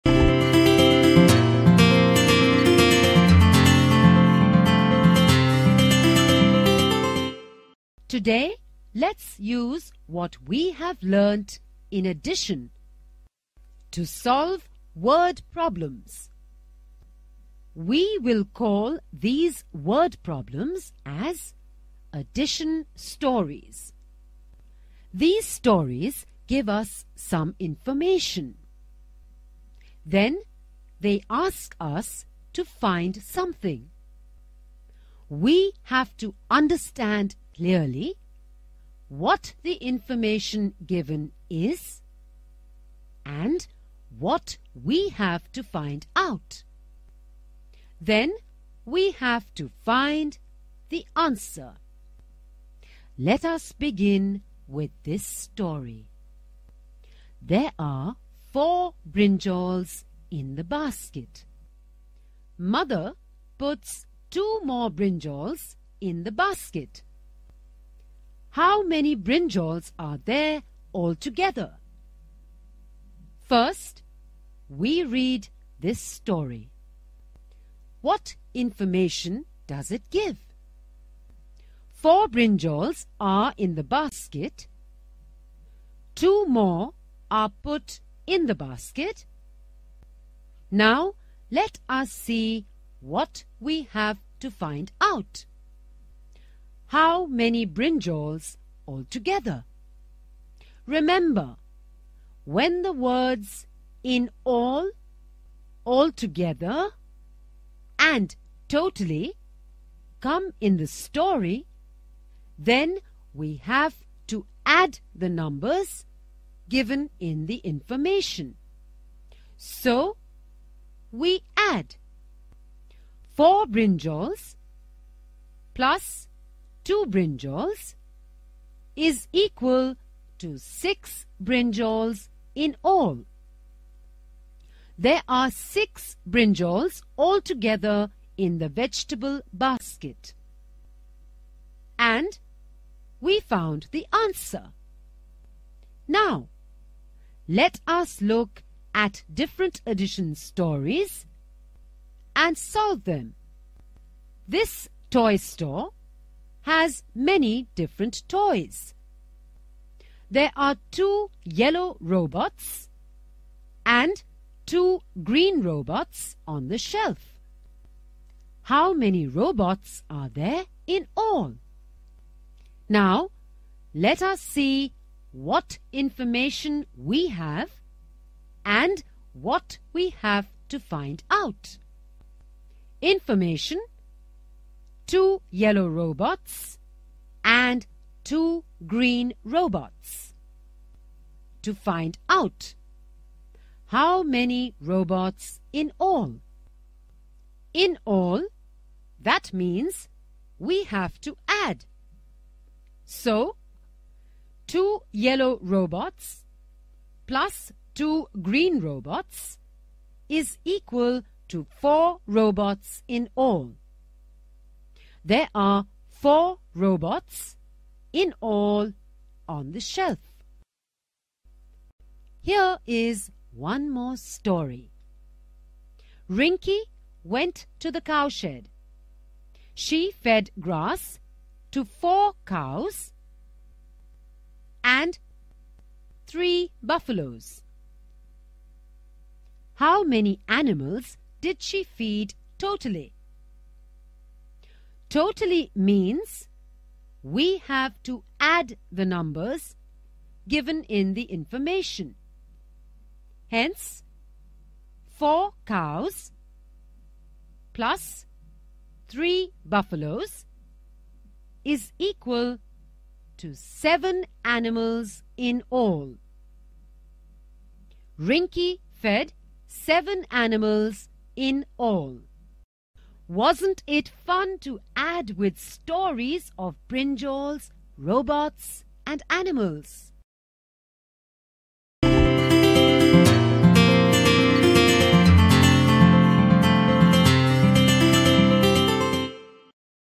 Learn Addition With The Help Of Addition Stories | Mathematics Grade 1 | Periwinkle
Hi Kids,
In this video we will learn how to add and get the answer, when we are given a story.